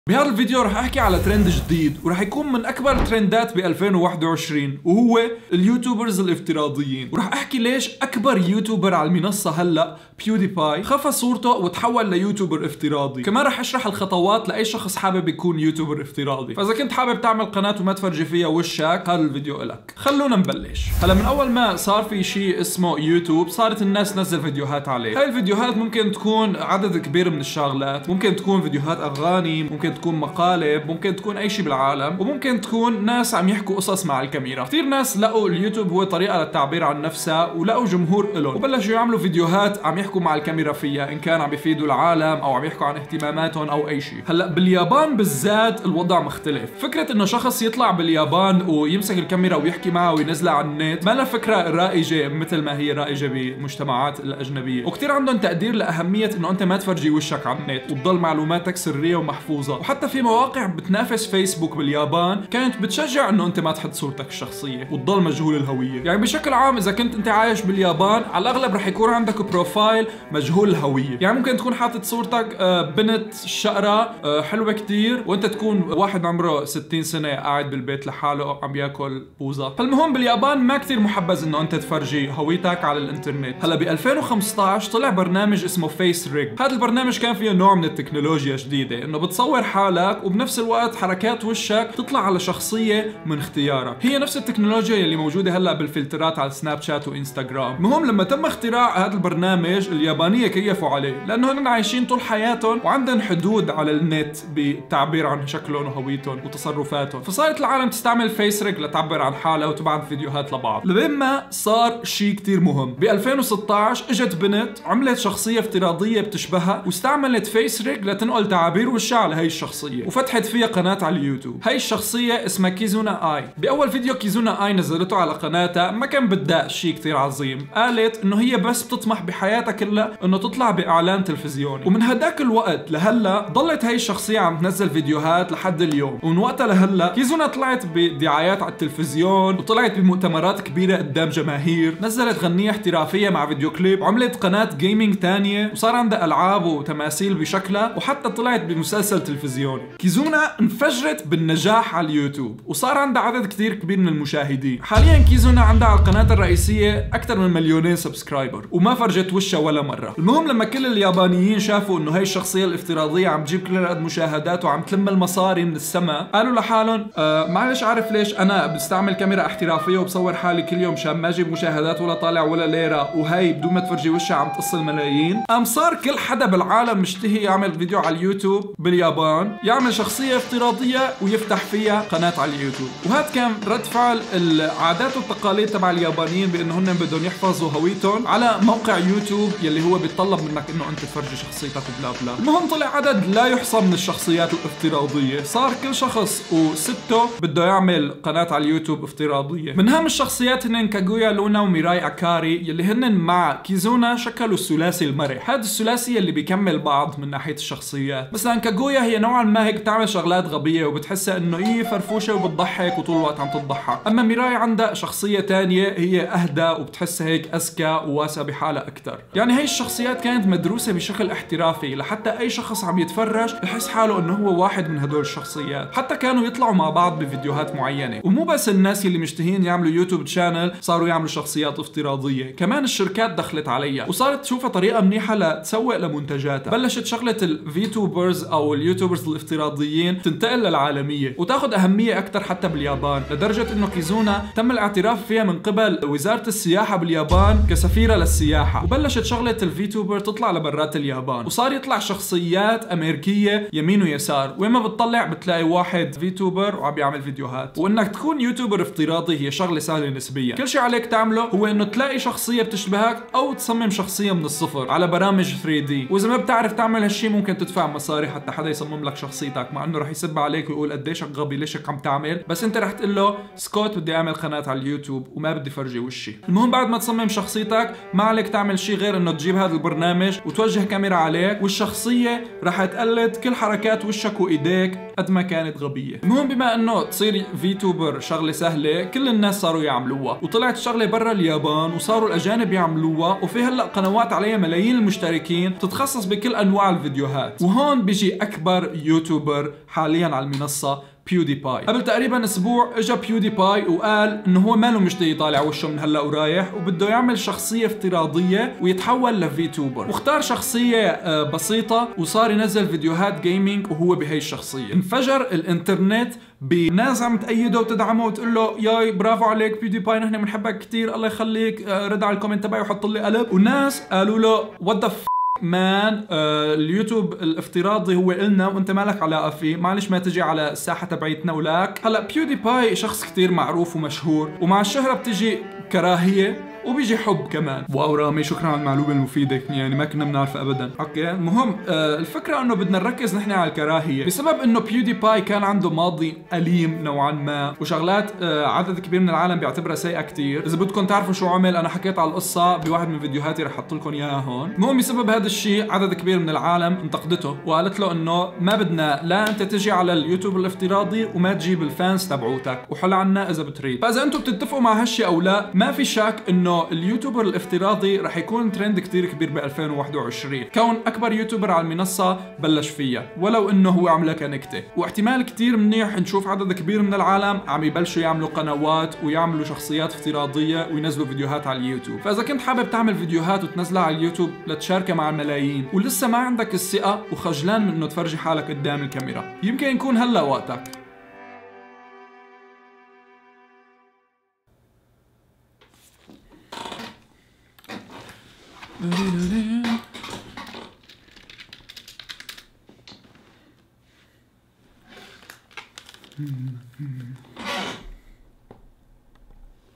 جهزوا حالكن لأكبر ترند باليوتيوب في 2021
اليوتيوبرز الافتراضيين Virtual Youtubers صاروا ترند على اليوتيوب وفتحوا قنوات ألعاب، ضحك، فلوق وكل أنواع القنوات. بهاد الفيديو رح نحكي عاليوتيوبر الافتاراضي وأكبر يوتيوبر عالمنصة بيوديباي